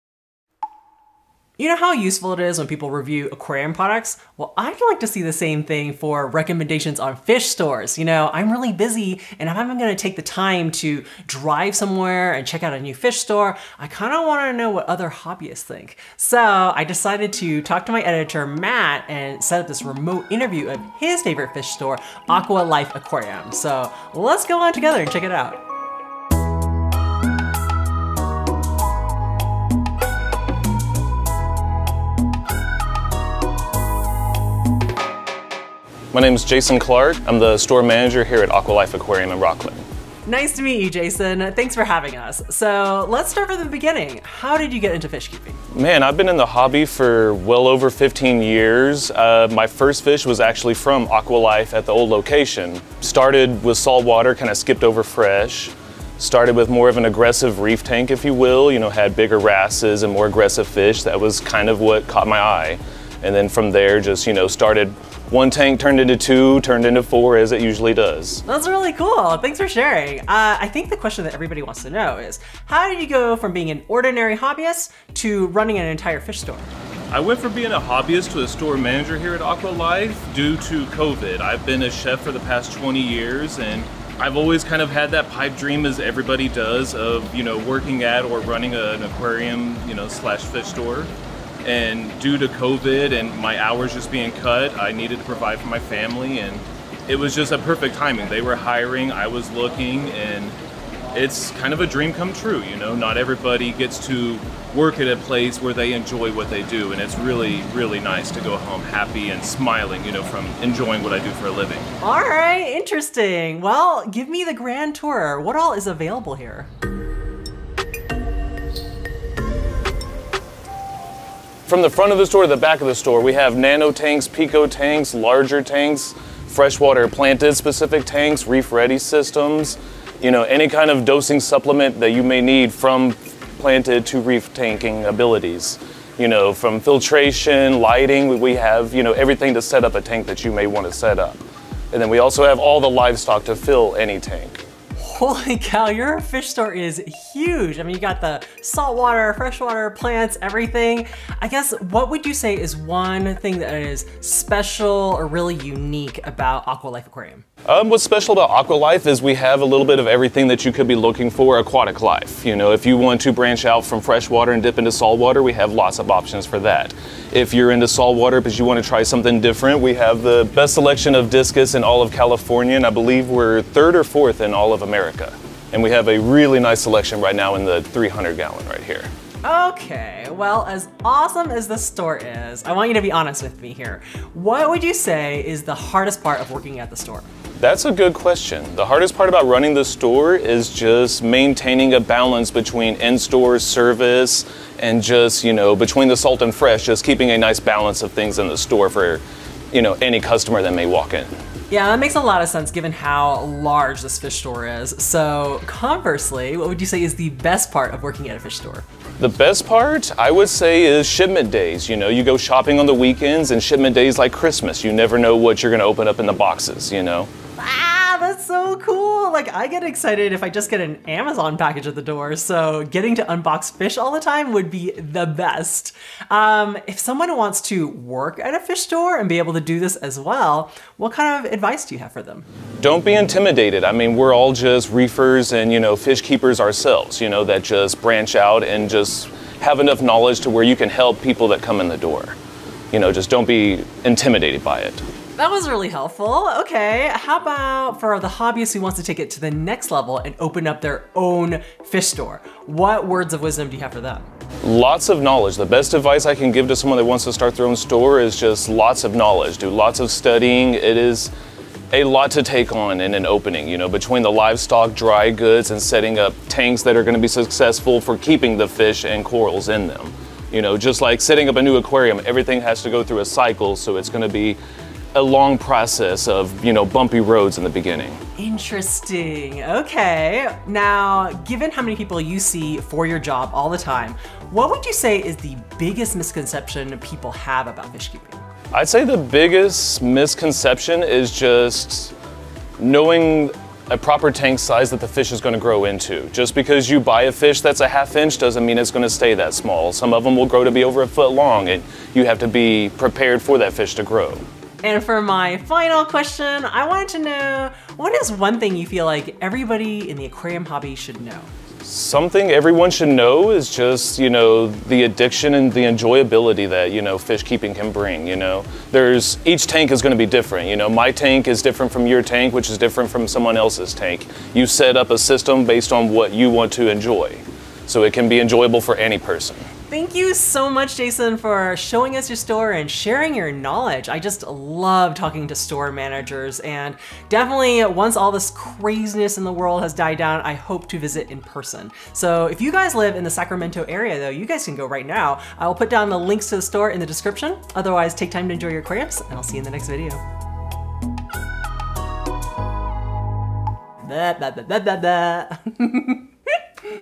I Found the BEST Discus Fish Store in California
It’s been a while since I’ve interviewed an aquarium hobbyist or professional on this channel, especially with the craziness of the past 2 years. However, my editor Matt helped me set up a remote interview with the store manager at his favorite local fish store (LFS). Thank you to Jason and Aqua Life Aquarium for your time! Please visit them in the greater Sacramento area and let me know what you think.

0:00 Meet Jason Clark from Aqua Life Aquarium
0:42 How did you get into fishkeeping?
1:10 How did you become a fish store manager?
1:53 Quick tour of Aqua Life Aquarium
2:31 What is unique about your fish store?
2:59 Hardest part of being a fish store manager
3:25 Best part of being a fish store manager
3:46 Advice on how to get a job at a fish store
4:07 Advice for hobbyists who want to open a fish store
4:41 Biggest misconception in the aquarium hobby
5:04 #1 thing every fishkeeper should know